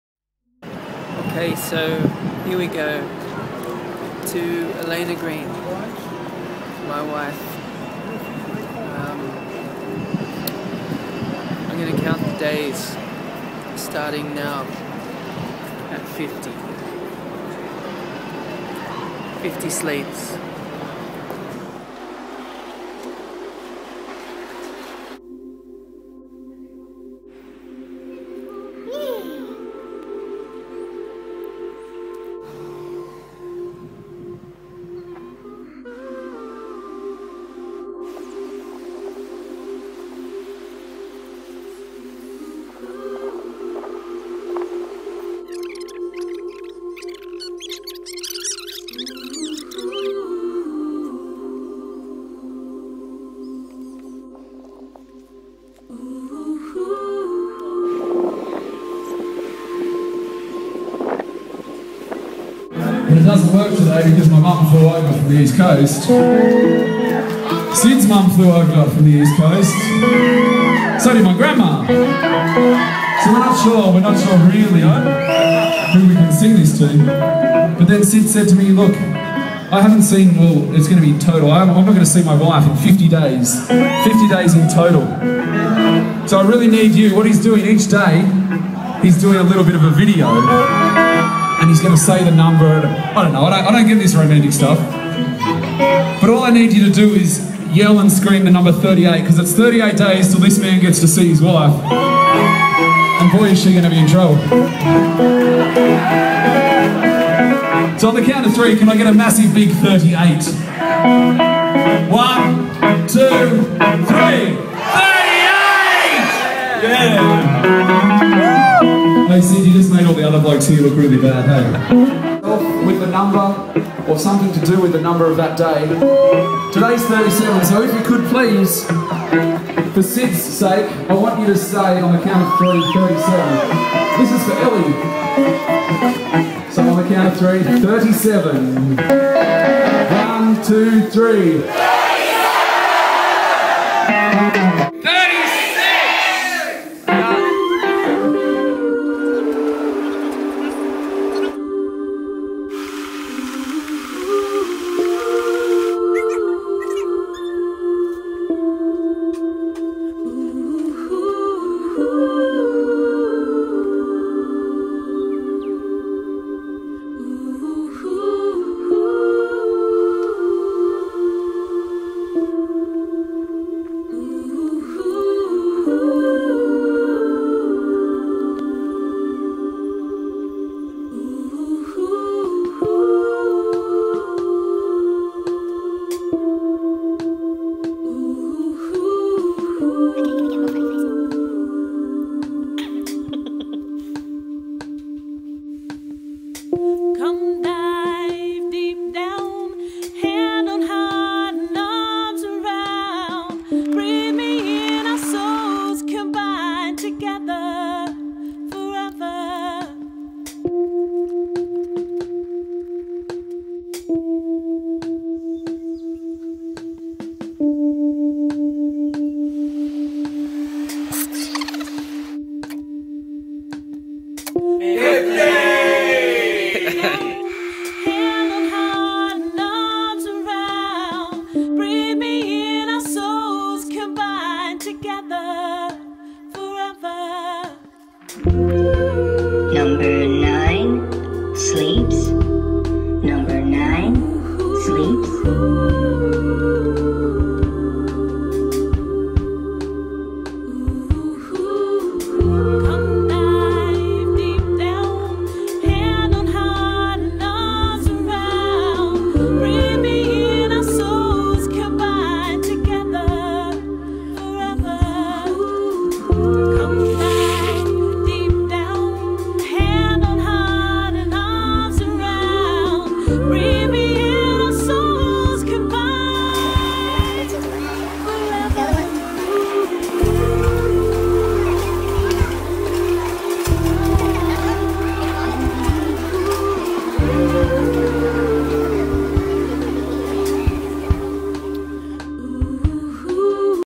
50 Sleeps - Syd & Elhi
Syd counts down the days until he unites with Elhi again.